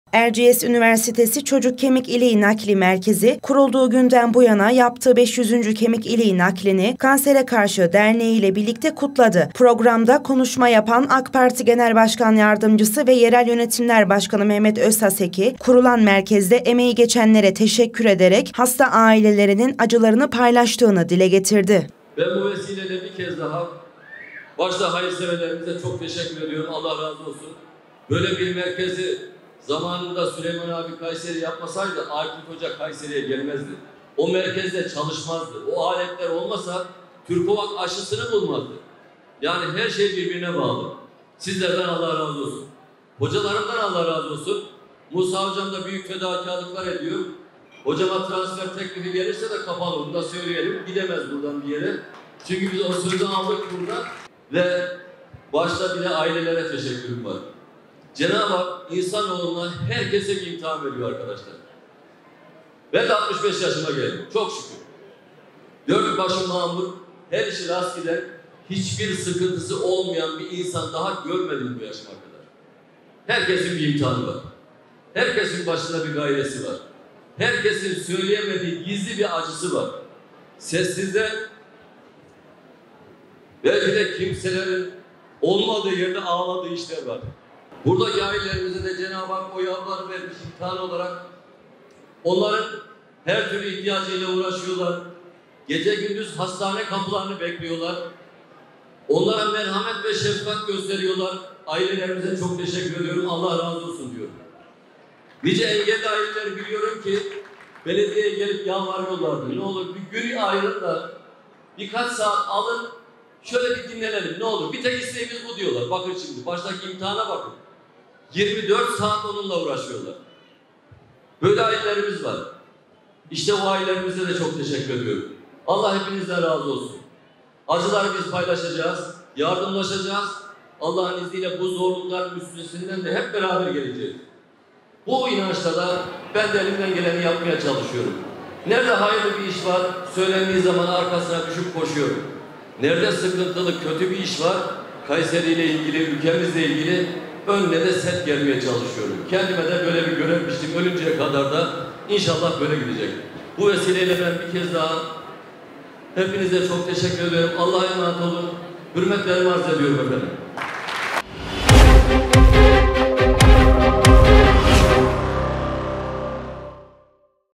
Özhaseki: “Ailelerimizin acılarını paylaşacağız, üstesinden geleceğiz”
KAYSERİ (1HA) -  Erciyes Üniversitesi (ERÜ) Çocuk Kemik İliği Nakli Merkezi kurulduğu günden bu yana yaptığı 500. Kemik iliği naklini; Kansere Karşı Birlikte (KANKA) Derneği ile birlikte kutladı. Programda konuşma yapan AK Parti Genel Başkan Yardımcısı ve Yerel Yönetimler Başkanı Mehmet Özhaseki, kurulan merkezde emeği geçenlere teşekkür ederek, hasta ailelerinin acılarını paylaştığını dile getirdi.
Kutlamaya daha öncesinde nakil olup iyileşmiş hastalar ve aileleri, protokol, hayırseverler, hastane personelleri, KANKA Derneği yönetim kurulu, bağışçıları ve gönüllüleri katılım sağladı. Düzenlenen programda konuşma yapan AK Parti Genel Başkan Yardımcısı ve Yerel Yönetimler Başkanı Mehmet Özhaseki ise yaptığı konuşma sırasında duygusal anlar yaşadı.

‘CENABI HAK, BURADAKİ AİLELERE O YAVRULARI İMTİHAN OLARAK VERMİŞ’

Hasta ailelerinin her koşulda çocuklarıyla ilgilendiğini ve bundan memnuniyet duyduğunu dile getiren Özhaseki, “Başta hayırseverlerimize çok teşekkür ediyorum. Böyle bir merkezi zamanında Süleyman hoca yapmasaydı Akif hoca Kayseri’ye gelmezdi. O merkezde çalışmazdı. O aletler olmasa TURKOVAC aşısını bulamazdı. Her şey birbirine bağlı, sizlerden Allah razı olsun. Hocalarımızdan da Allah razı olsun. Musa hocam da büyük fedakarlık yapıyor. Ailelere teşekkürüm var. Cenabı hak herkesi imtihan ediyor. Herkesin söyleyemediği gizli bir acısı var. Sessizler, belki de kimseleri olmadığı yerde ağladığı şeyler var. Buradaki ailelerimize de cenabı hak o yavruları vermiş imtihan olarak. Onların her türlü ihtiyacıyla uğraşıyorlar, gece gündüz hastane kapıları bekliyorlar, onlara merhamet ve şefkat gösteriyorlar. Ailelerimize çok teşekkür ediyor, Allah razı olsun diyorum. Nice engelli aileler biliyorum ki belediyeye geldiği zaman yalvarıyorlar, ne olur bir gün ayırın da birkaç saat alın biraz dinlenelim tek isteğimiz bu diyorlar. İşte böyle bir imtihan 24 saat bu durumla uğraşıyorlar. İşte bu ailelerimize çok teşekkür ediyoruz. Acılarını paylaşacağız, yardımlaşacağız ve Allah’ın izniyle bu zorlukların üstesinden geleceğiz. Bu inançla ben elimden geleni yapmaya çalışıyorum” diye konuştu.